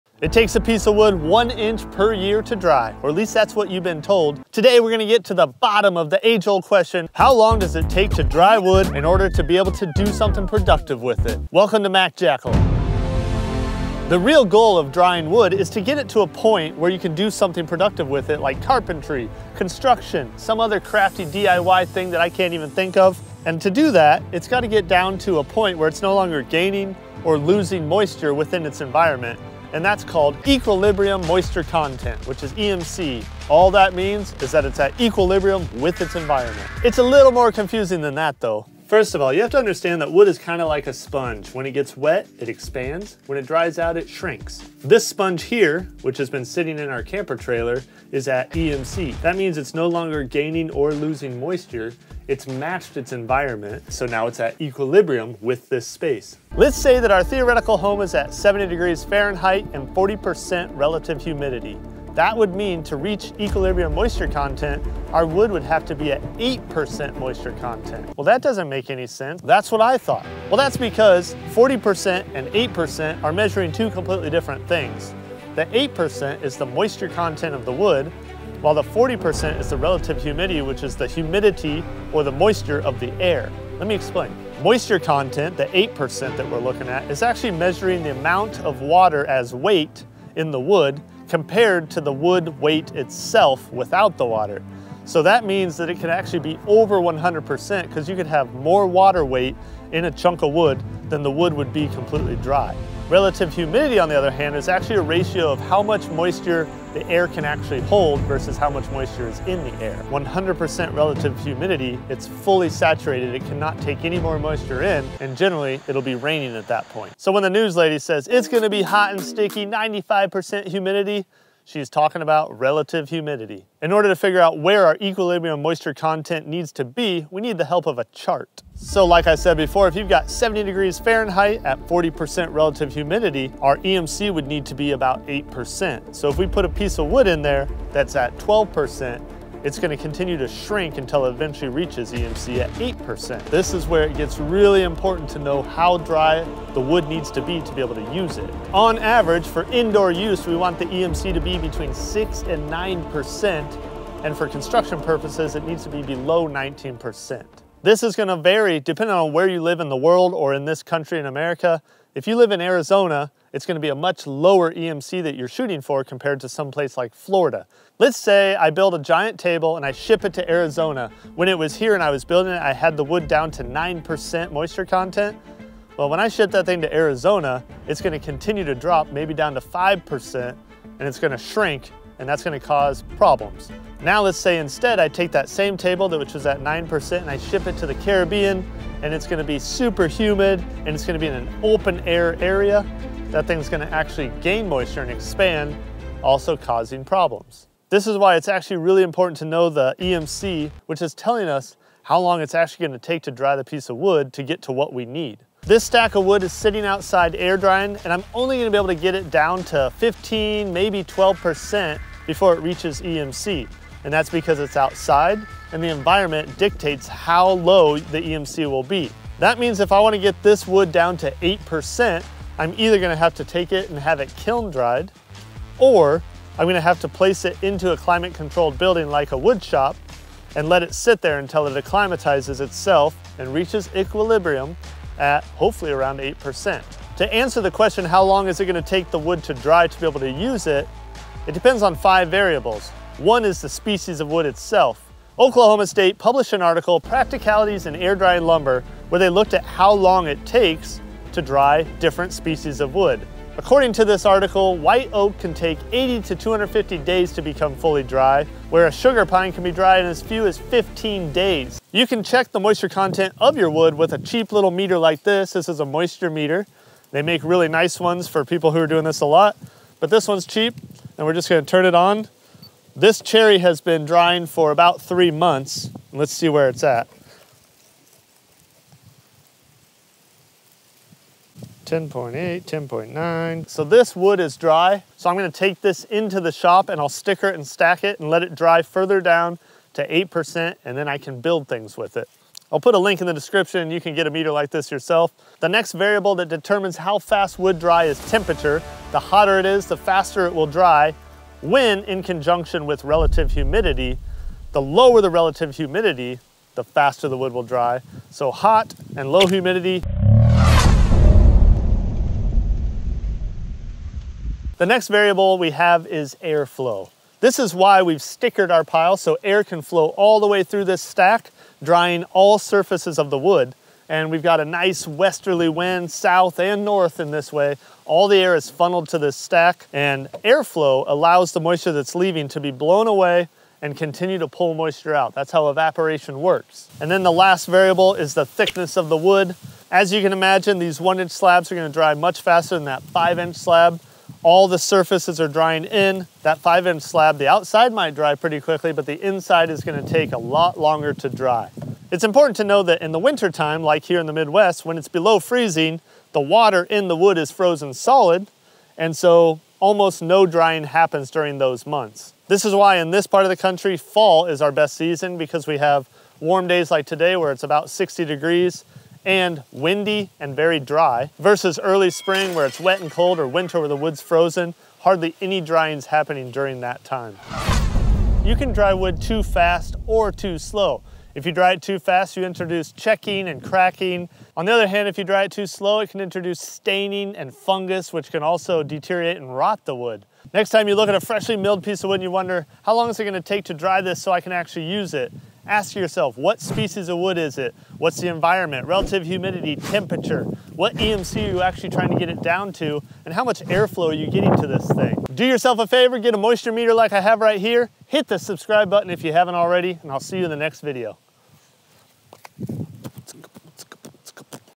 Can You Dry Wood Faster?
We've been told it takes 1 year per inch to air dry wood. That's not quite true. So how long does it take wood to dry once it's been milled and stacked?

Moisture Meter: